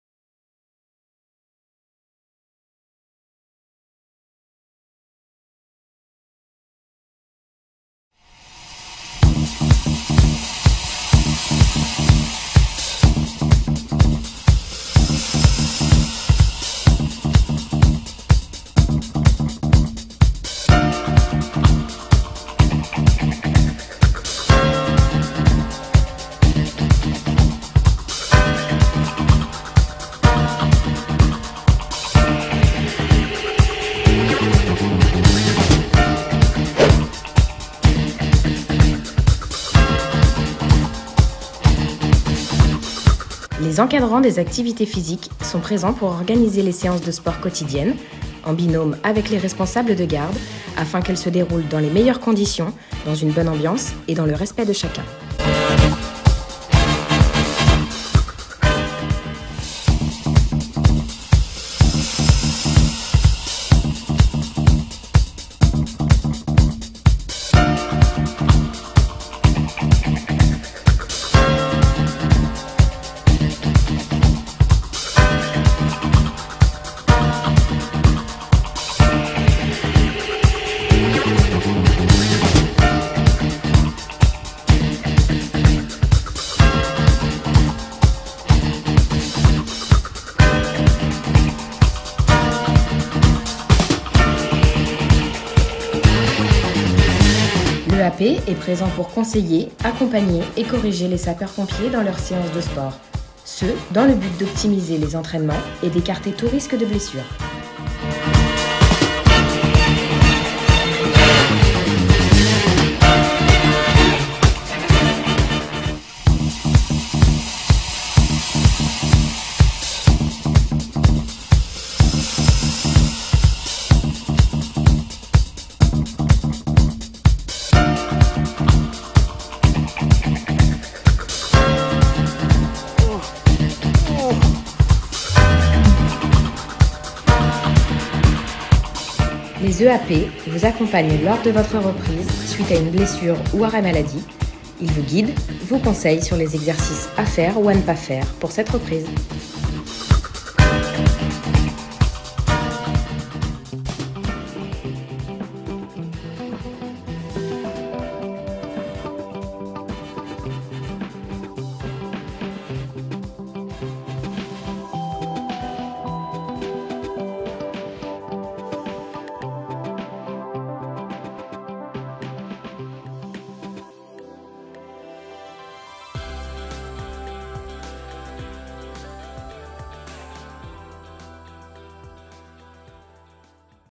Présentation des EAP 2021
Les activités physiques sont essentielles dans la vie des sapeurs-pompiers pour leur bien-être mais aussi pour leur sécurité.

Les AP faut pas les zapper